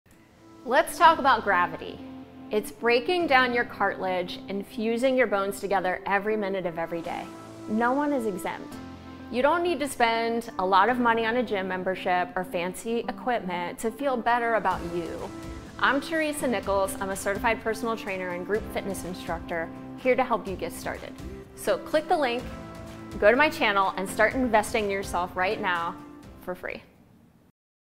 Gravity
No one is exempt from the forces of gravity; even if you think you are in "good shape" and you don't need to exercise. Your muscles want and need to be challenged in different ways. If you sit at a desk for hours at a time, do the Telemotion routines between tasks or meetings. If you drive a lot, do the routines between stops. Invest in your body daily.